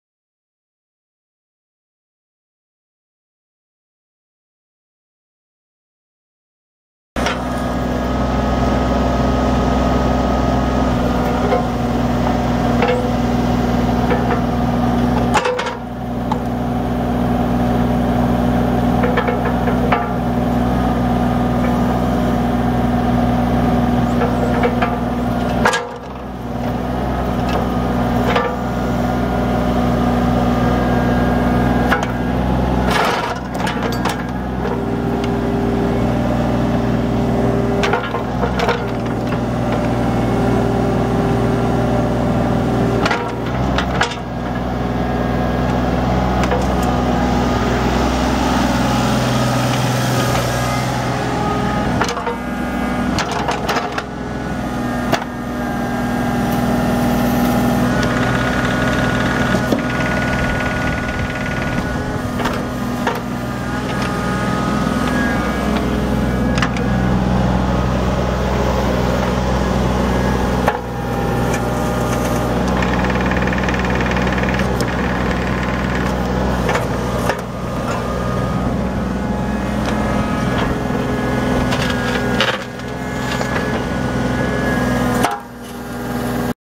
2004 Kubota U35 compact excavator for sale | sold at auction June 26, 2014
SOLD! Tri-State Bobcat Business Moving Auction

2004 Kubota U35 compact excavator
3,353 hours on meter
Kubota three cylinder diesel engine
Hydrostatic transmission
Two speed travel
OROPS
Joystick controls
Pattern changer
Boom lights
Glow plugs
53" stick length
Variable angle boom
Mechanical quick coupler
Hydraulic thumb
Needs one new fitting

18"W bucket with side cutters
Four bolt-on teeth
67"W backfill blade
Four bottom rollers
12"W rubber tracks
8,025 lbs. base operating weight
Thumb needs one new fitting
Serial 20554

It was located in Hudson, WI, and we sold it to the highest bidder for $18,700.